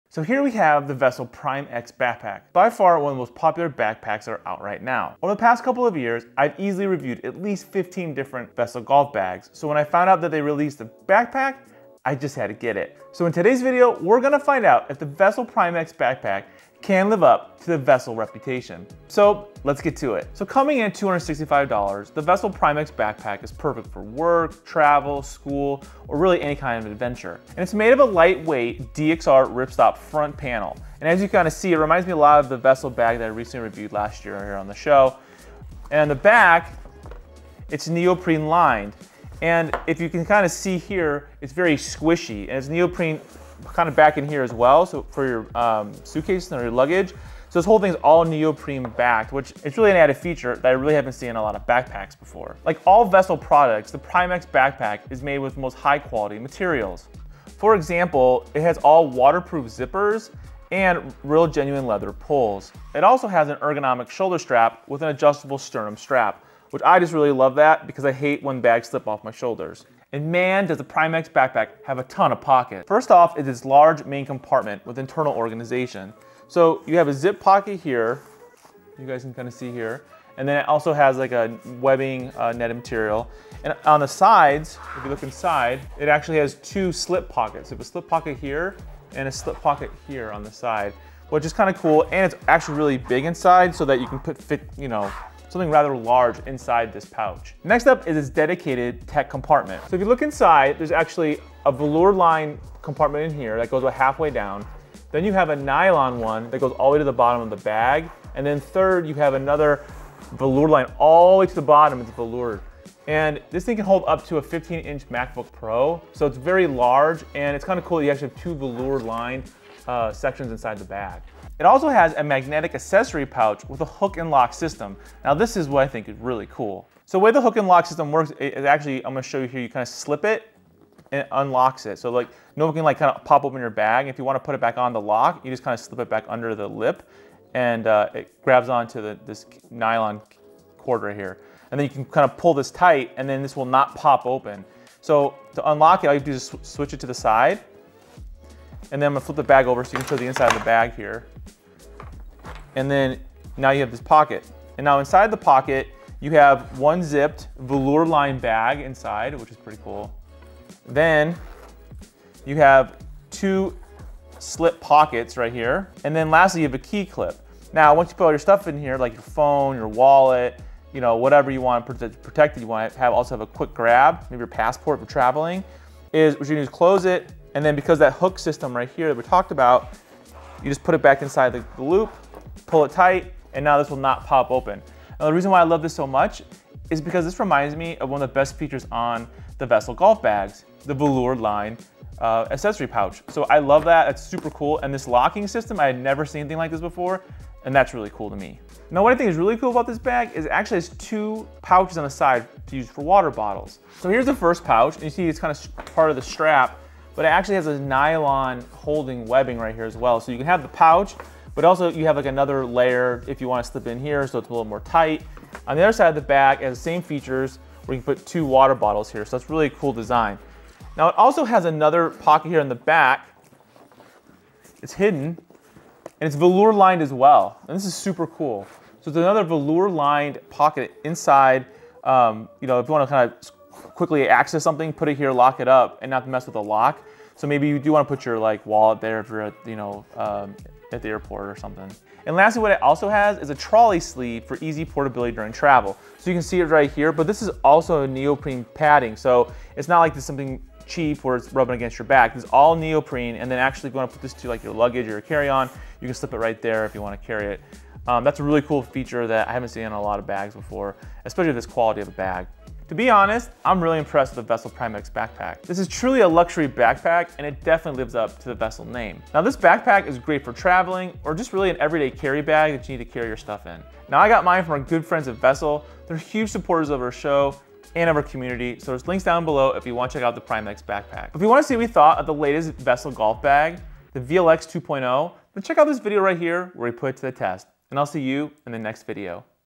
This INSANE Backpack Has a Huge Secret ... Vessel PrimeX
Hey Guys, in todays video I am checking out the Vessel PrimeX Backpack to see if it is really worth it.

1️⃣ Vessel PrimeX